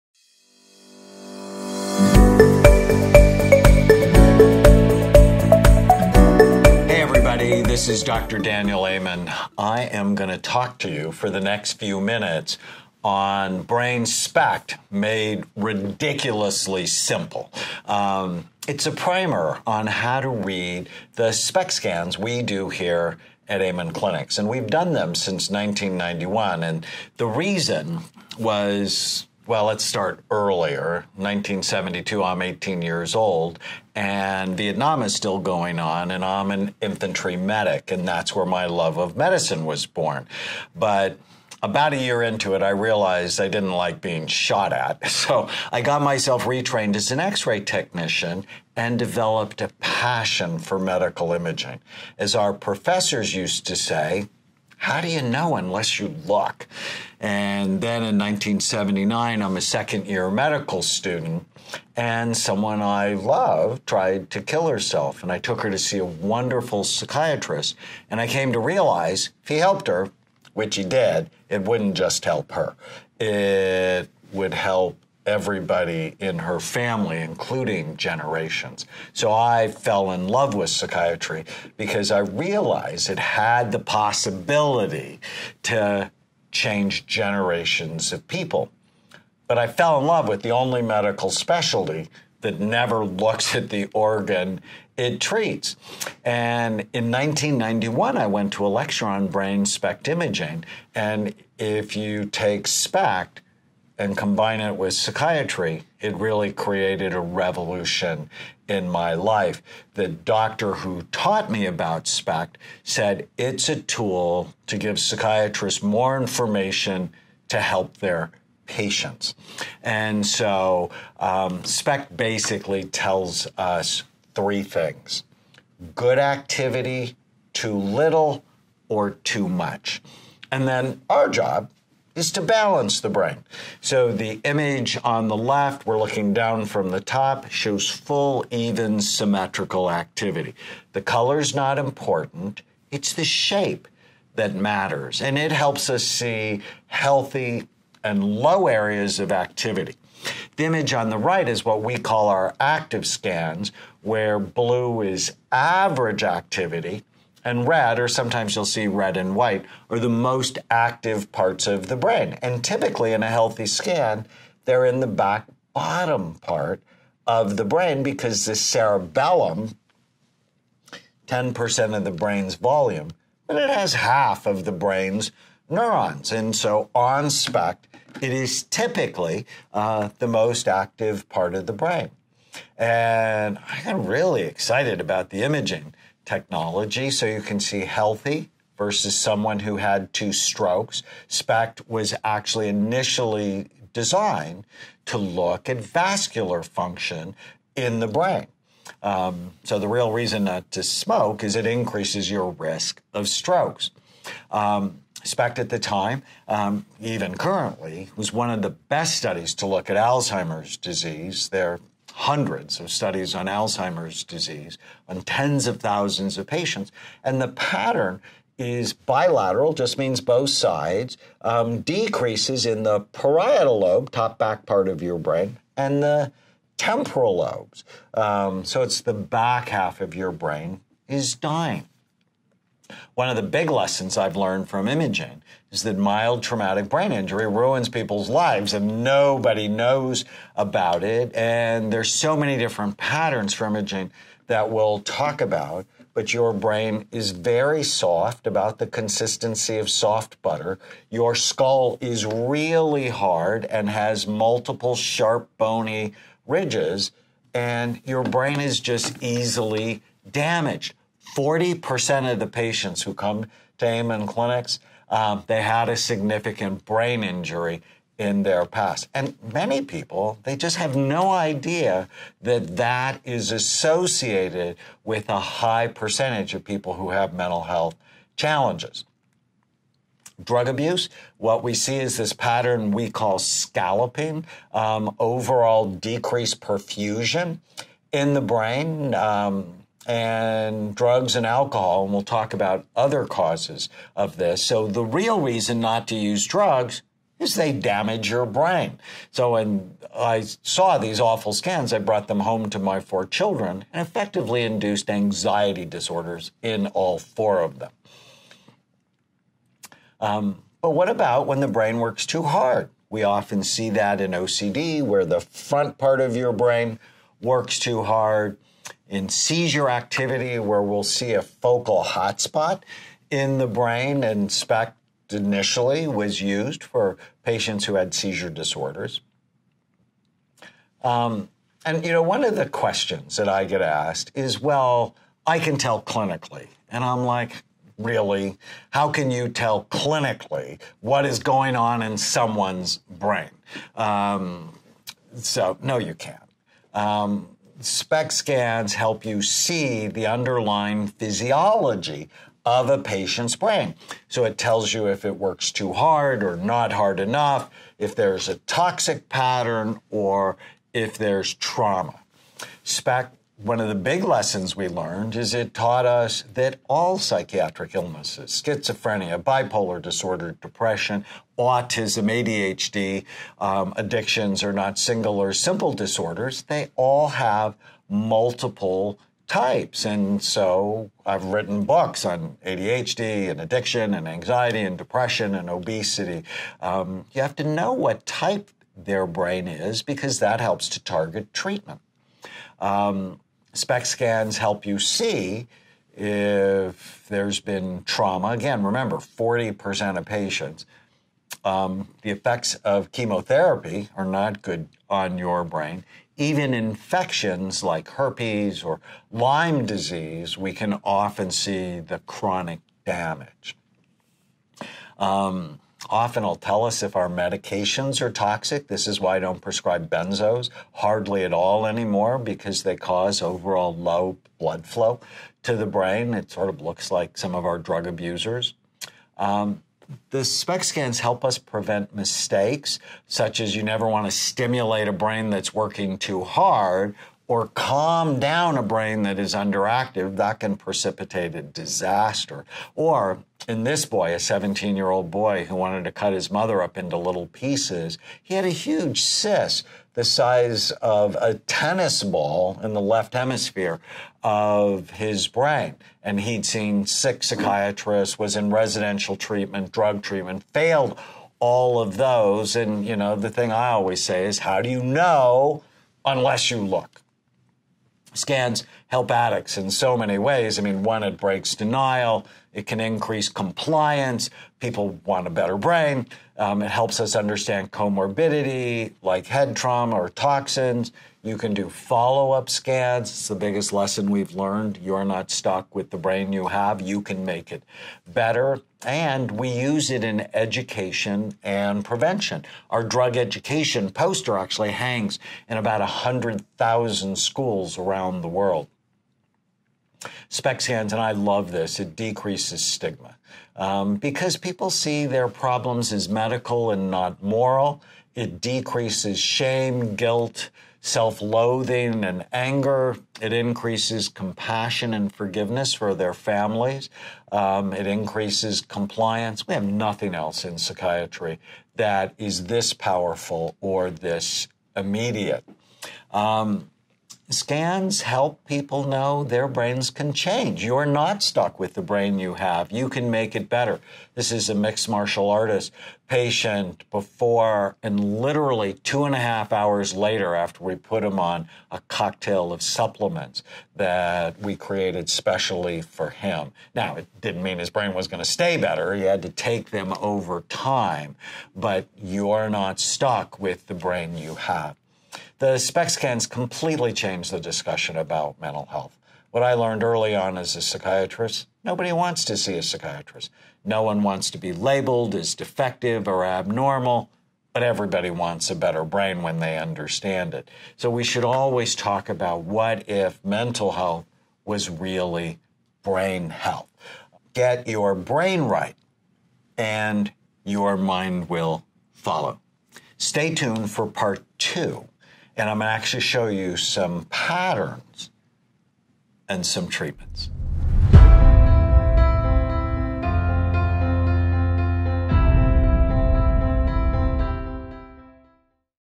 SPECT Made Ridiculously Simple (Part 1) | Dr. Daniel Amen
What is a SPECT scan, and why are people calling it the future of psychiatry?

In this short video, America's most popular psychiatrist Dr. Daniel Amen explains the origins of his work with brain SPECT imaging and how it changed psychiatry forever.

Dr. Amen shows how a brain SPECT scan can be used to see characteristics in the brain such as a particular type of ADD, a previously unknown brain injury, or a depression pattern that may require a particular type of medication.

0:00 Introduction
2:15 HEALTHY BRAIN SPECT SCANS
3:08 HEALTHY VS. 2 STROKES
3:38 HEALTHY VS. ALZHEIMER'S DISEASE
4:03 HEALTHY VS. TRAUMATIC BRAIN INJURY
4:44 HEALTHY VS. DRUG ABUSE
5:14 HEALTHY VS. OCD
5:19 HEALTHY VS. SEIZURE ACTIVITY
5:50 SPECT SCANS HELP TO SEE THE UNDERLYING PHYSIOLOGY IN INDIVIDUAL PATIENTS
6:17 SPECT SCANS HELP TO SEE ALL PSYCHIATRIC ILLNESSES HAVE DIFFERENT TYPES
6:36 SPECT SCANS HELP TO SEE TRAUMATIC BRAIN INJURIES, TOXINS, INFECTIONS
6:58 SPECT SCANS HELP TO SEE HOW MANY CURRENT MEDS CAN BE TOXIC TO BRAIN FUNCTION
7:14 SPECT SCANS HELP TO PREVENT MISTAKES, SUCH AS...
7:51 SPECT SCANS HELP ADDICTS
8:55 SPECT SCANS HELP PATIENTS KNOW THEY CAN CHANGE THEIR BRAIN
9:22 SPECT SCANS COMPLETELY CHANGE THE DISCUSSION AROUND MENTAL HEALTH